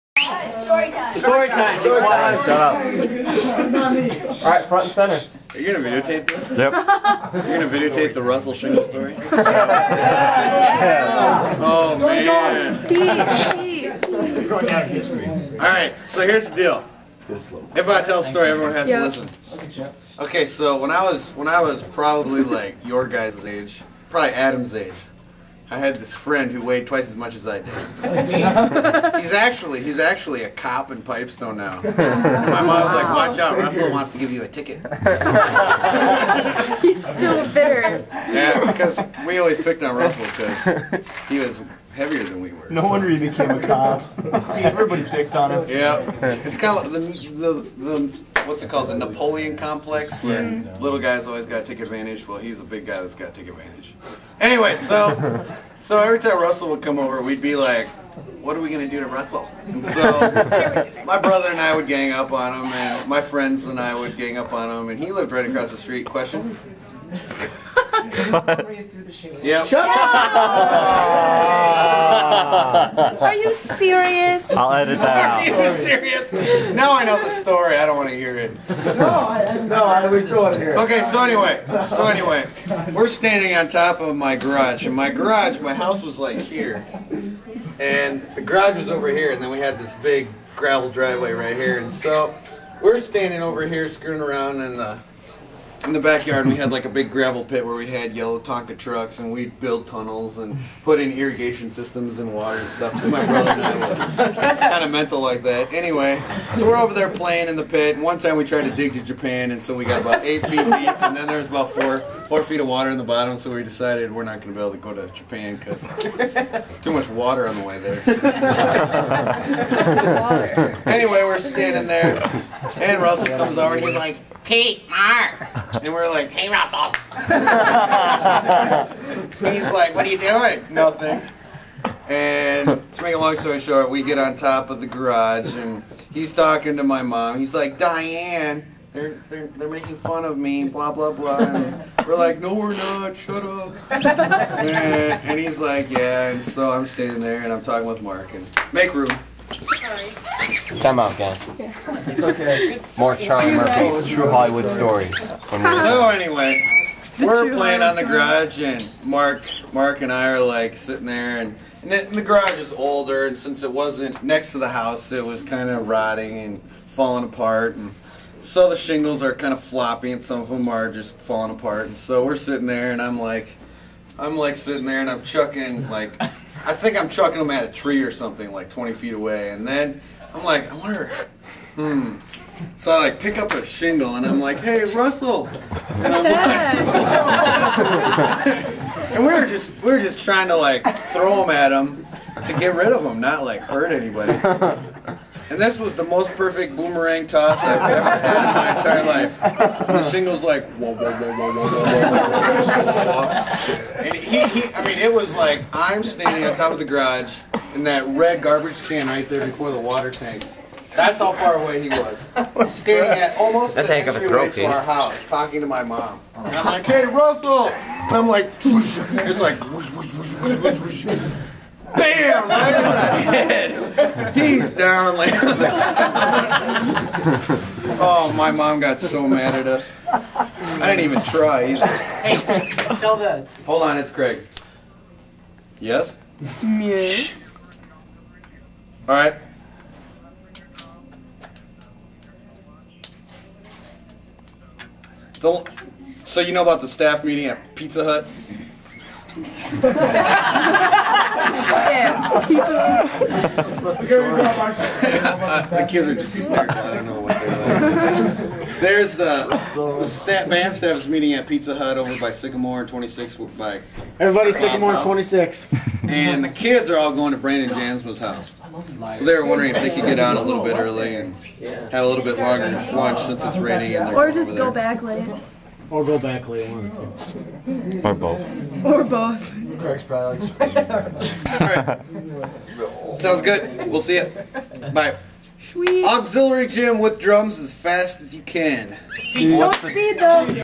LHS Drumline - Russel Shingle Story
A rainy day story. Pete telling the "Russel Shingle Story" to the 2005-06 LHS Drumline.